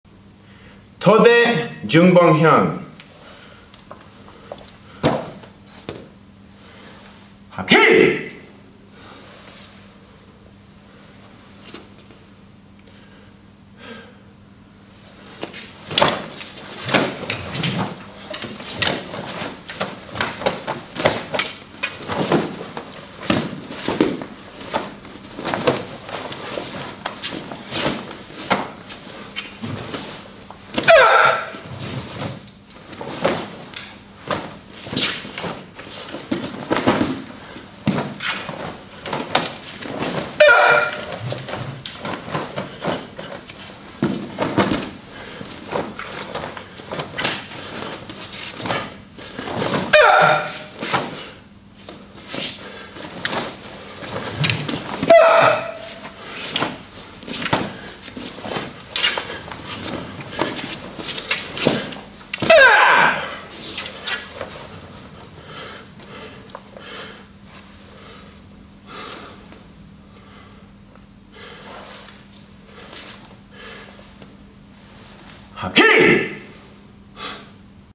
Todae Joong Bong Hyung- Foundation Mid- Staff Form
Todae Joong Bong Hyung- Foundation Mid- Staff Form performed by Master Justin Castillo. This form is a black belt form used regionally by the Foundation Hapkido & Self-Defense Association.